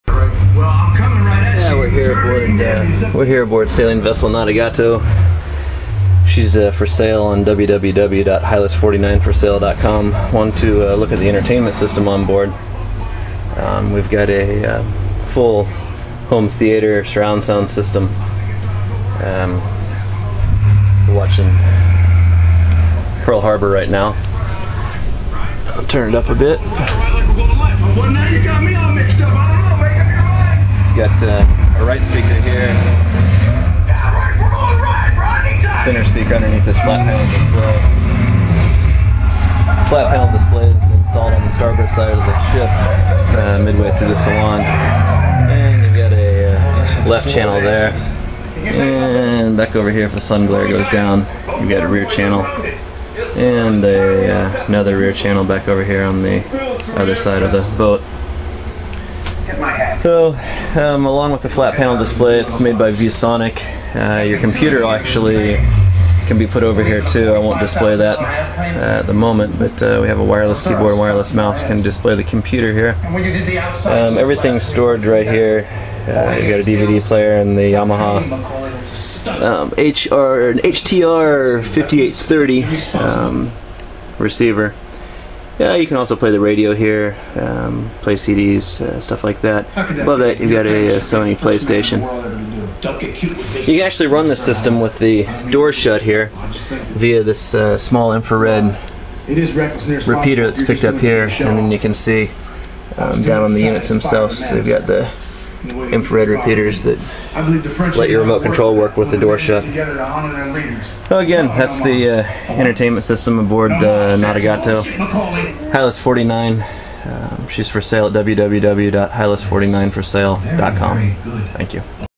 Virtual Survey and Tour - Hylas 49 Nadagato Entertainment and Home Theater
Here you can take a look at the home theater system aboard Nadagato.  We have built a flat panel display into the boat and added a Yamaha receiver to power a 6 speaker system.  Not mentioned in the video, there is a sub which is hidden in the boat.